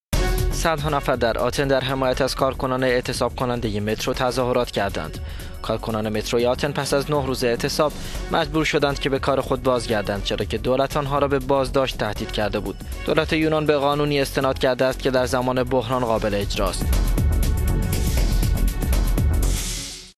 تظاهرات در آتن در حمایت از کارگران اعتصابی
صدها نفر در آتن در حمایت از کارکنان اعتصاب کننده مترو تظاهرات کردند.

کارکنان متروی آتن پس از نه روز اعتصاب مجبور شدند که به کار خود بازگردند چرا که دولت آنها را به بازداشت تهدید کرده بود.

دولت به قانونی استناد کرده بود که در زمان بحران از جمله در زمان جنگ یا حوادث طبیعی قابل اجرا است.